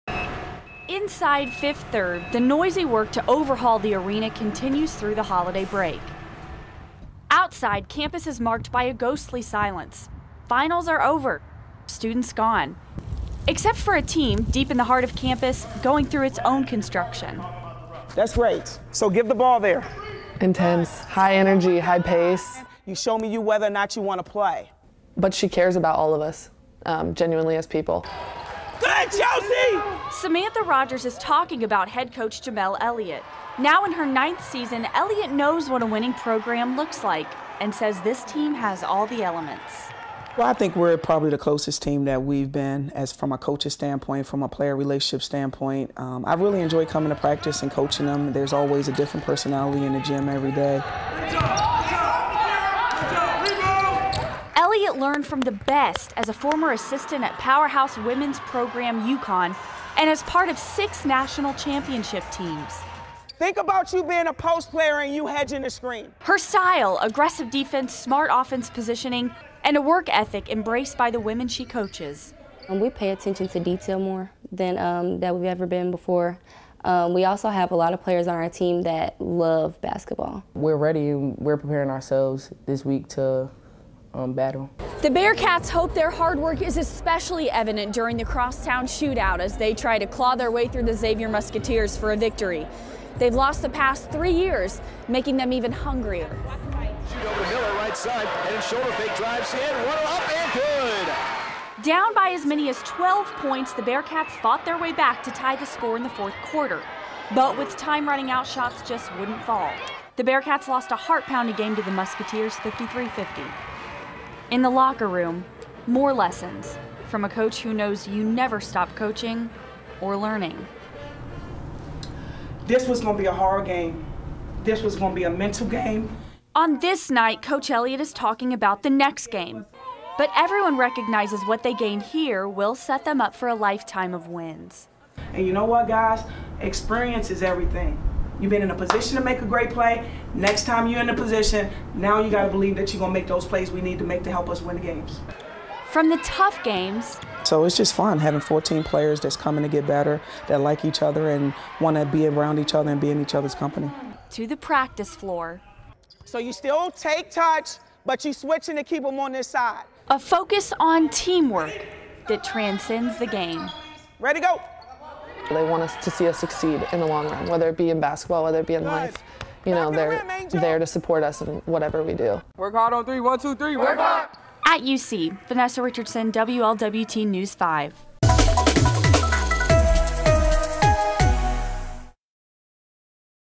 Lady Bearcats adapt to venue change among makeover
Jemelle Elliott coaches for ninth season​